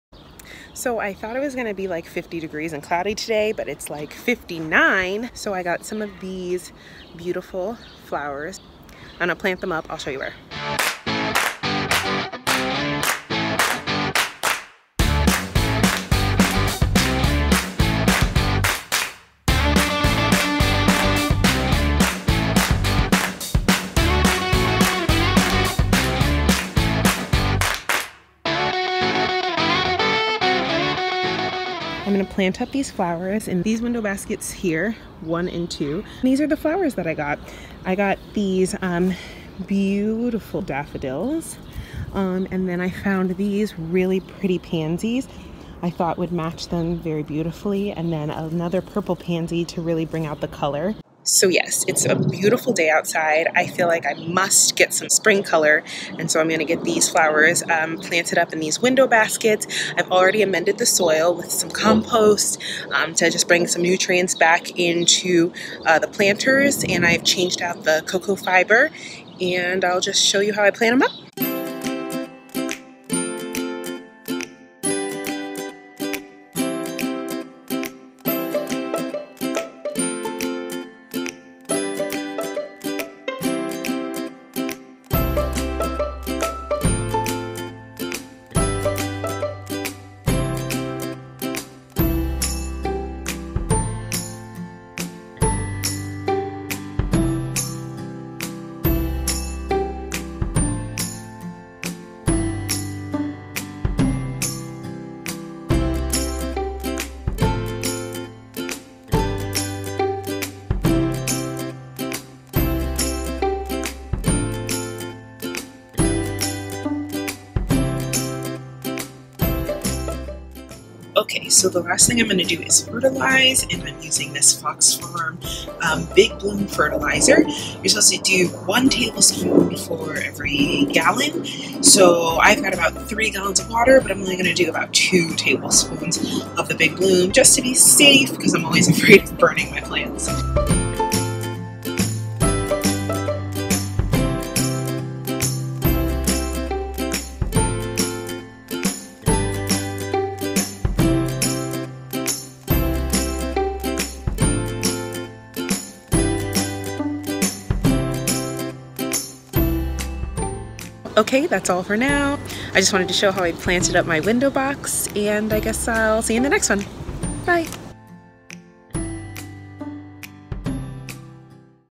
Daffodils in a Window Basket
I plant pansies and daffodils in my spring window basket, and II mentioned in my April Garden Tour that I would share how I put it together on my porch.

They are still doing really well, despite the daffodils being dead headed.

Any suggestions on summer plants for the baskets? Im looking to pant things that keep flies away.